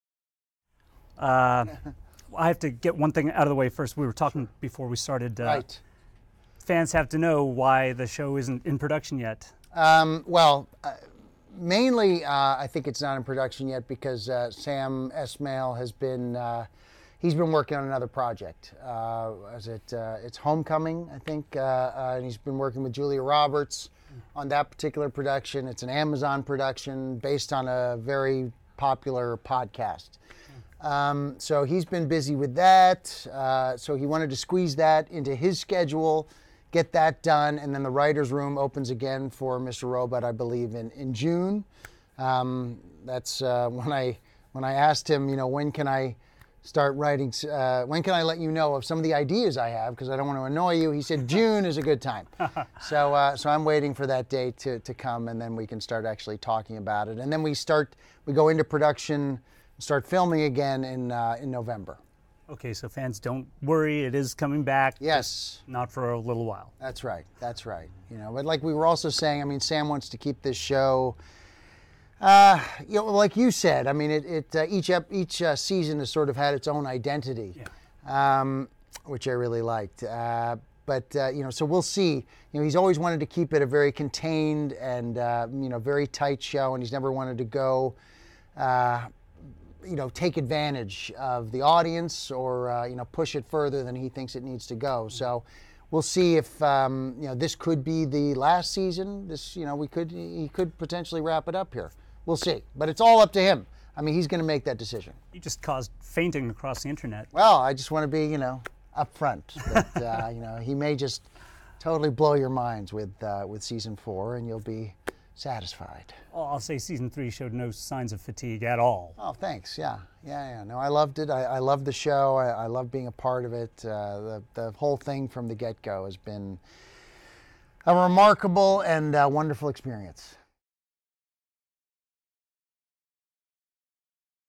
Don’t worry, ‘Mr. Robot’ fans – it’s coming back. Christian Slater says so
The actor has ideas ready to go for show creator Sam Esmail when the writers room reconvenes – and he adds that the fourth season could be the last.